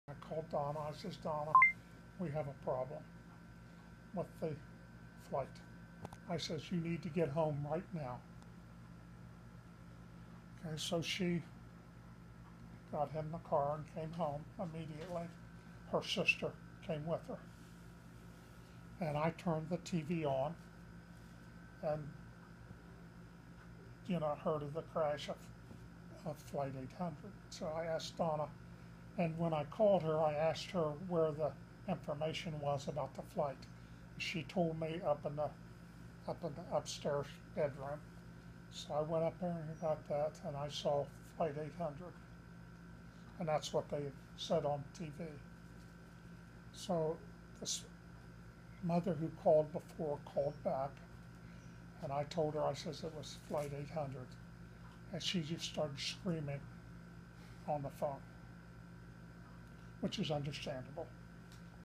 Don Nibert recounts the death of his daughter
Don Nibert talks about the day his daughter Cheryl Nibert, 16, a French Club student at Montoursville High School, died in the crash of Flight 800 off Long Island, New York, July 17, 2016. Sixteen students and five chaperones died on their way to Pa